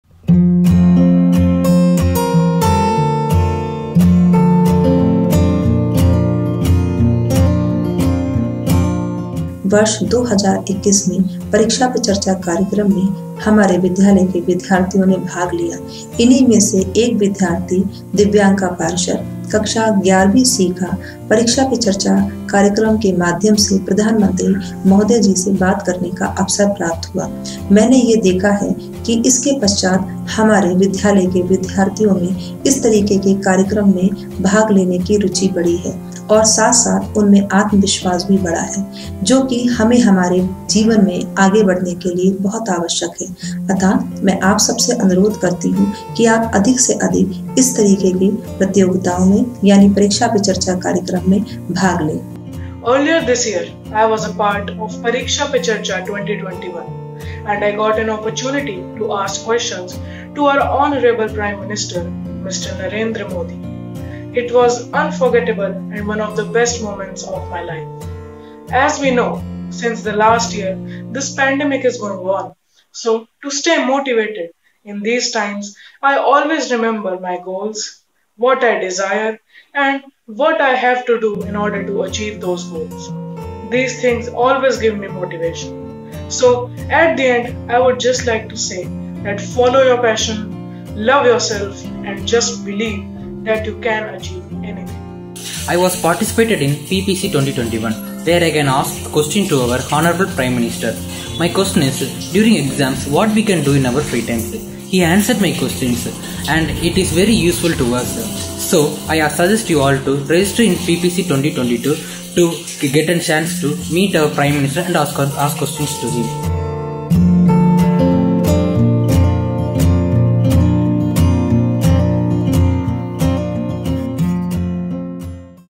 प्रधानमंत्री से संवाद के बाद बढ़ा आत्मविश्वास
पीएम के साथ परीक्षा पे चर्चा संवाद में भाग लेने के बाद छात्रों का बढ़ा मनोबल और उनमें हुआ ऊर्जा का संचार! 2022 संवाद में भाग लेने के लिए तुरंत पंजीकरण करें